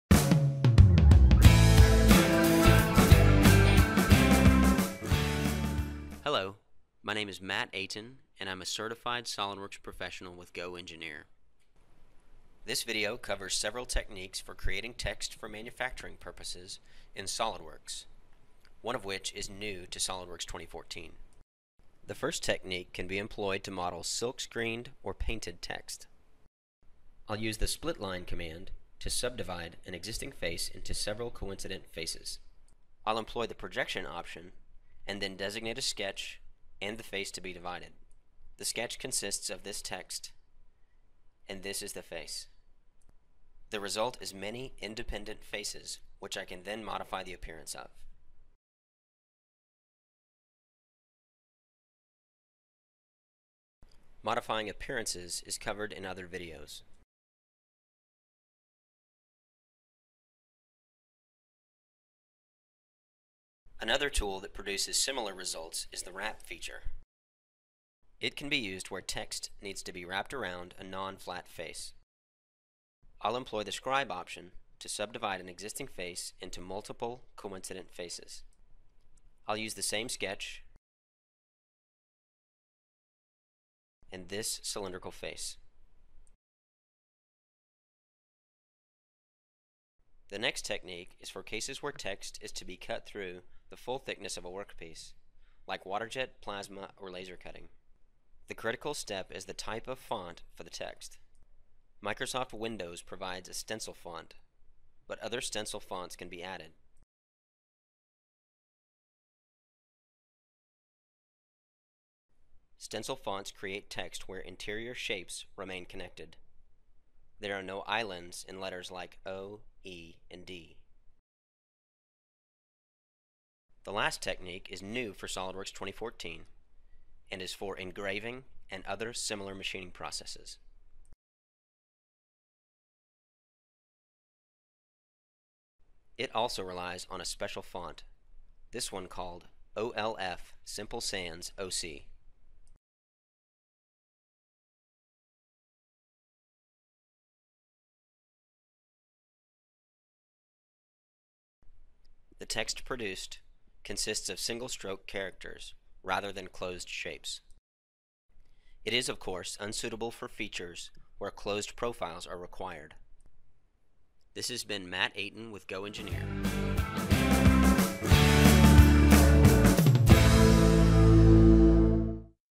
SOLIDWORKS 2014 – Text for Manufacturing
Learn about three techniques used with text in SOLIDWORKS models for specific manufacturing processes.  One of the techniques is new for SOLIDWORKS 2014.